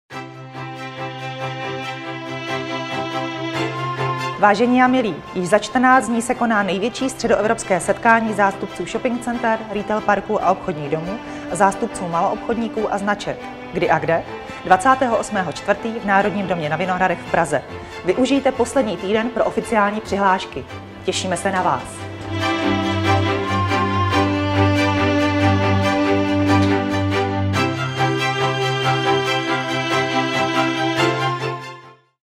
RTC2020 - Pozvánka
Videopozvánka na 17. ročník největšího setkání odborníků na problematiku retailu a shopping center, RetailCon 2020.